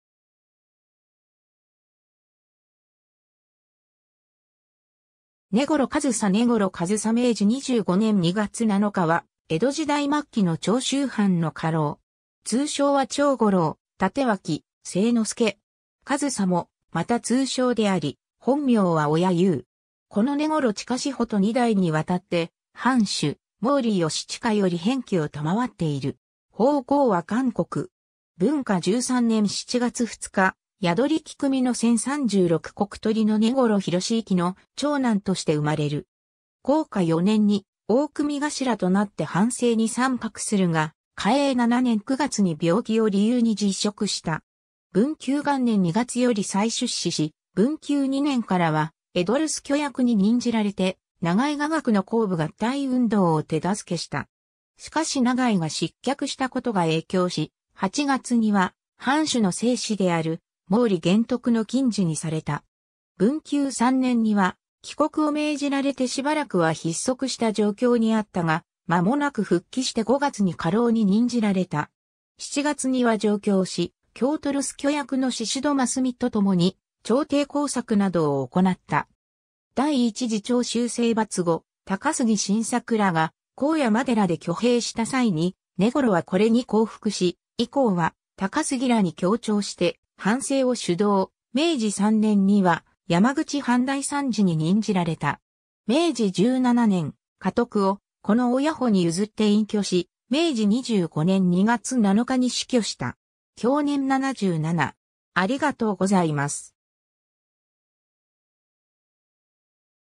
根来上総
根来上総 根来 上総（ねごろ かずさ、文化13年7月2日（1816年7月26日） - 明治25年（1892年）2月7日）は、江戸時代末期（幕末）の長州藩の家老。
通称は長五郎・帯刀・勢之祐。
「上総」もまた通称であり、本名は親祐（ちかすけ）。
子の根来親保（ちかやす）と二代に亘って、藩主毛利慶親より偏諱（「親」の字）を賜っている。
法号は甘谷。
 文化13年（1816年）7月2日、寄組（上士）の1,036石取りの根来煕行（ひろゆき、「煕」は毛利斉熙から賜った字）の長男として生まれる。
 弘化4年（1847年）に大組頭となって藩政に参画するが、嘉永7年（1854年）9月に病気を理由に辞職した。
 文久元年（1861年）2月より再出仕し、文久2年（1862年）からは江戸留守居役に任じられて長井雅楽の公武合体運動を手助けした。
しかし長井が失脚したことが影響し、8月には藩主の世子である毛利元徳の近侍にされた。
 文久3年（1863年）には帰国を命じられてしばらくは逼塞した状況にあったが、まもなく復帰して5月に家老に任じられた。
7月には上京し、京都留守居役の宍戸真澂と共に朝廷工作などを行なった。
 第1次長州征伐後、高杉晋作らが功山寺で挙兵した際に根来はこれに降伏し、以降は高杉らに協調して藩政を主導、明治3年（1870年）には山口藩大参事に任じられた。
 明治17年（1884年）、家督を子の親保に譲って隠居し、明治25年（1892年）2月7日に死去した。
享年77。